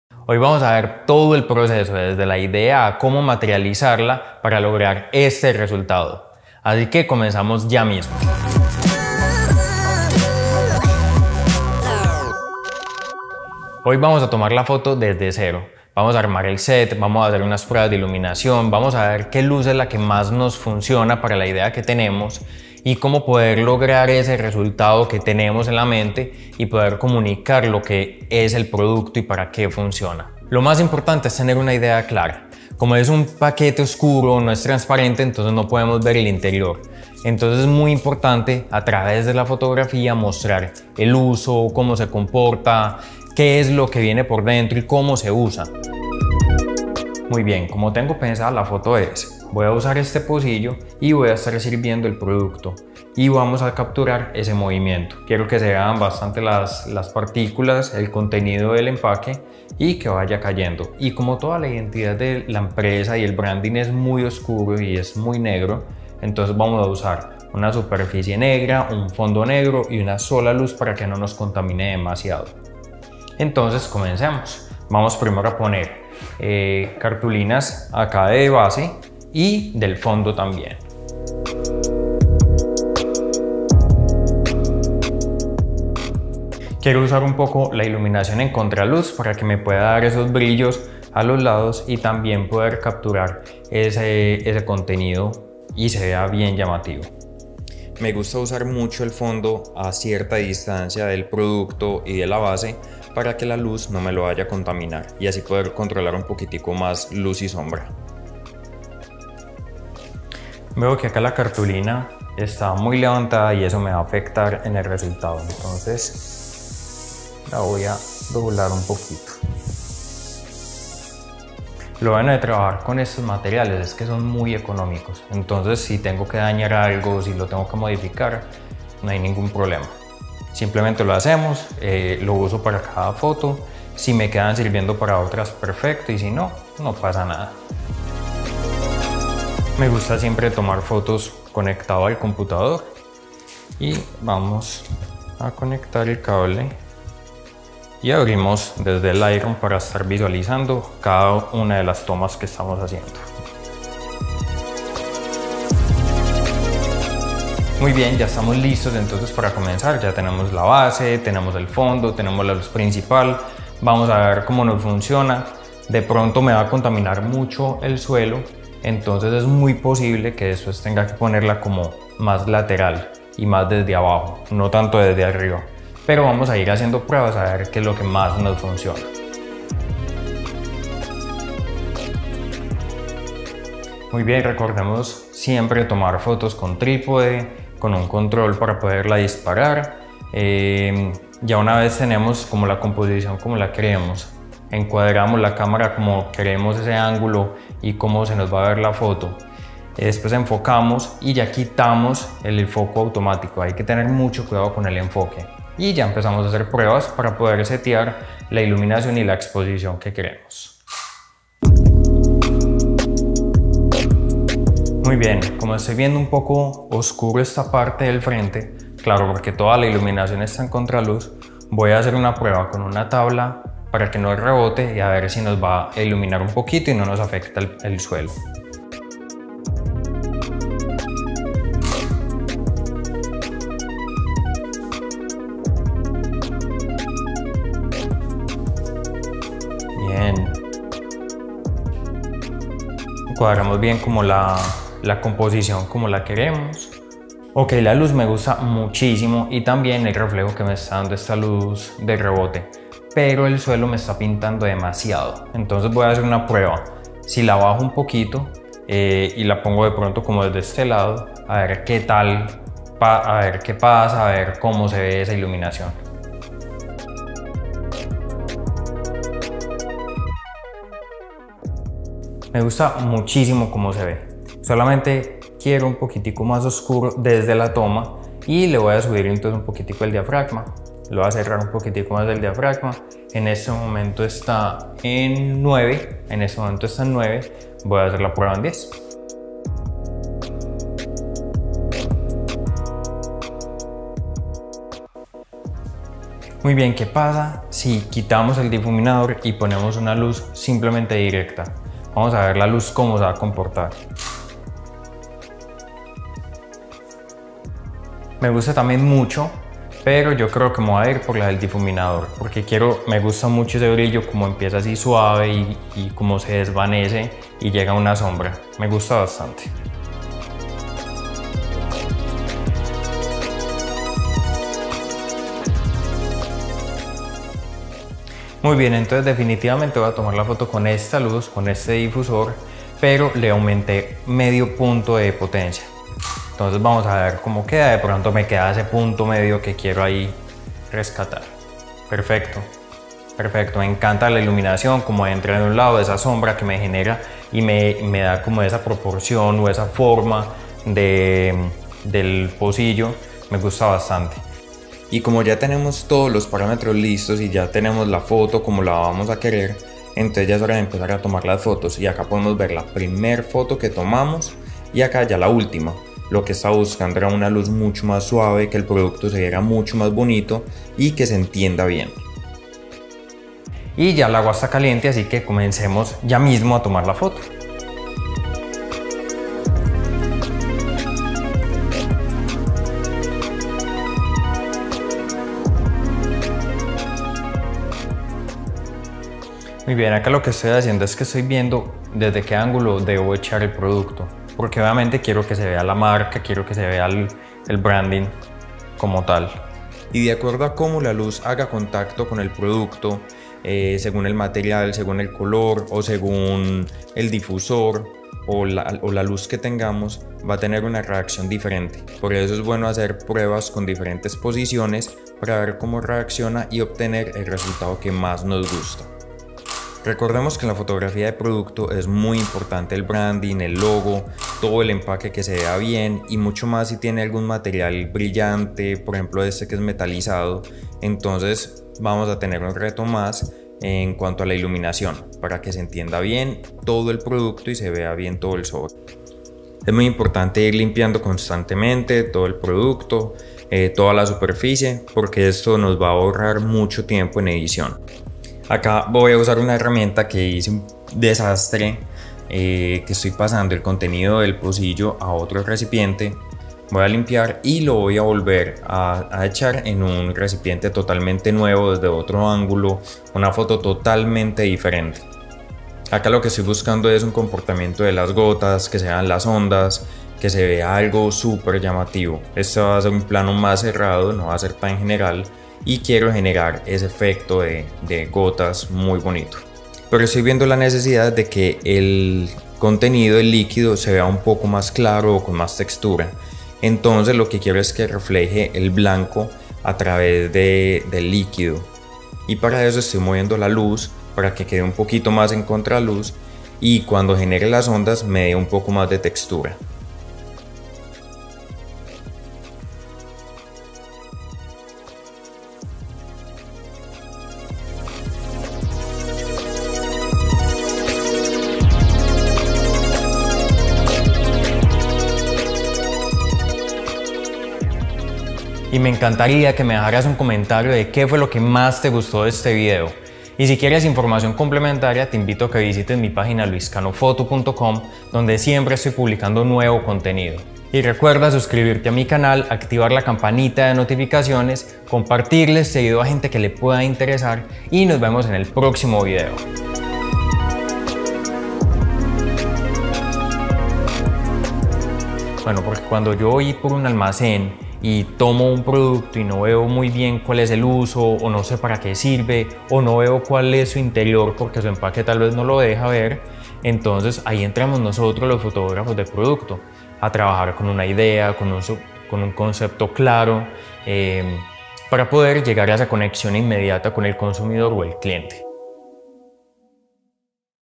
FOTOGRAFÍA COMERCIAL desde una IDEA💡
Como hacer fotografía de producto (PANELCAFÉ) comenzando desde un concepto💡 hasta el resultado final.
En este video vemos todo el proceso para lograr una imagen INCREÍBLE solucionando problemas de iluminación, composición, entre otros... Pero lo mas importante, CON UNA IDEA CLARA. ✔

y déjame un comentario de lo que más te gustó de este video.

Equipos de fotografía:
Camara D610
Lente 85mm

Equipos Vlog:
Camara Fuji XA5